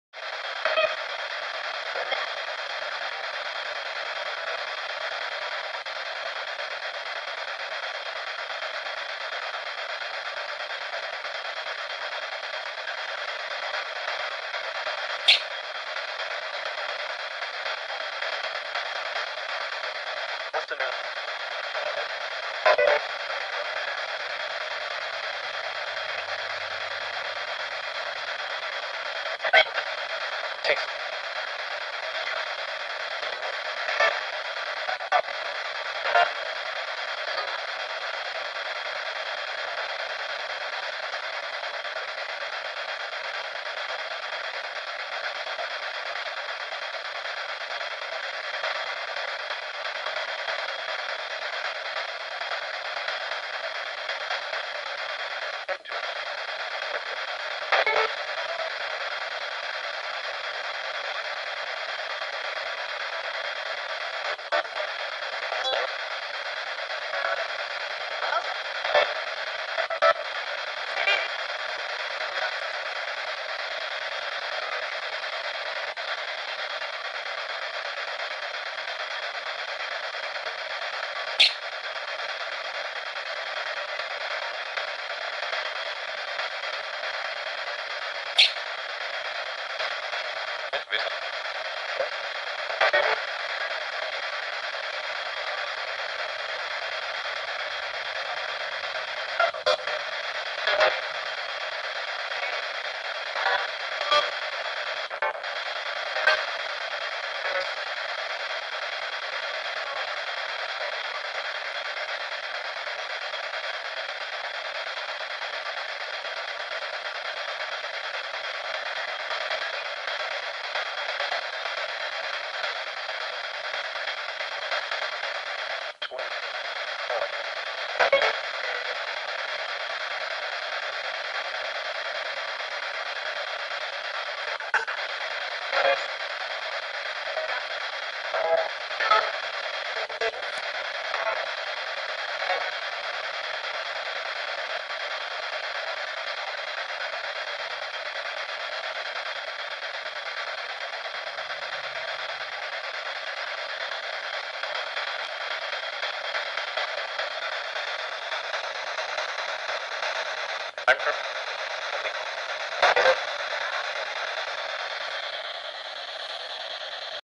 Ghost Box
Experimental video using the P-SB7 as seen on tv used by the tv crew Ghost Adventures, with the Ghost Radar App from Spudd Pickles for iPhone. RAW UNEDITED video using my Sony DSC-RX100 Camera.

Let me know if you catch anything interesting I haven't been catching anything through the ghost box yet or I just don't have the ears for it. I get exceptionally strong readings on my ghost radar app when used in conjunction with my ghost box. On the ghost radar app it kept spelling out FFFFOOO repeatedly I have no idea what it meant.

On the ghost radar app the words that were spoken BEHIND, PRESENT, TEN, THANK, POSSIBLY, STRENGTH, TRIBE, and FRIEND.